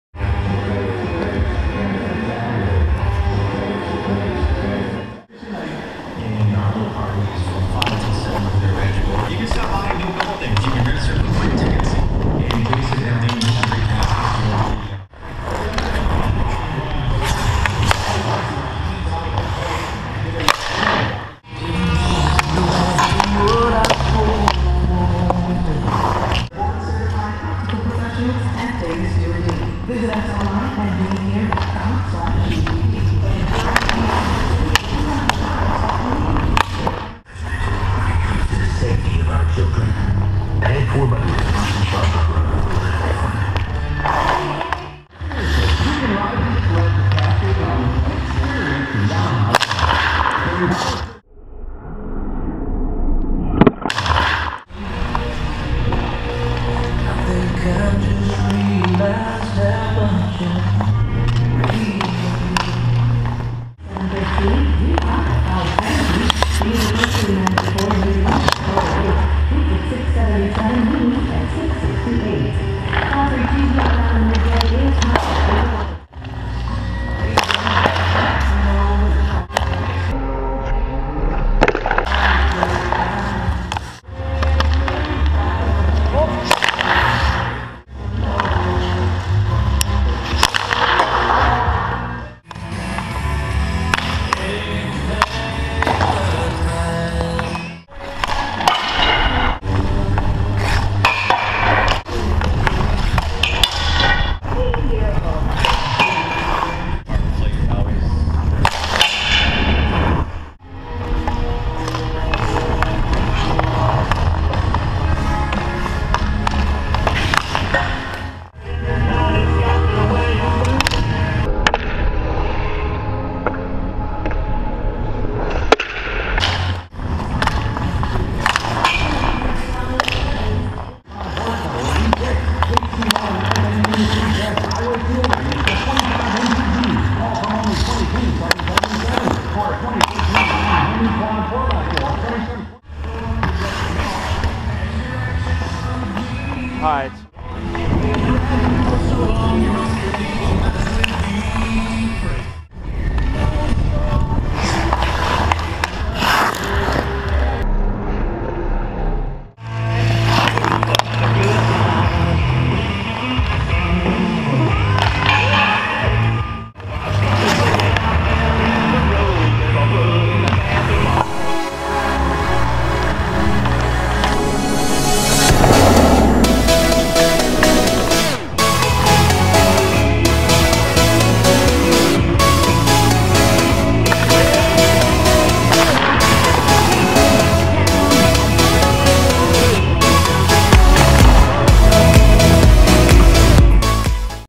Open Hockey-GoPro
Nothing special, testing the GoPro out on the ice at open hockey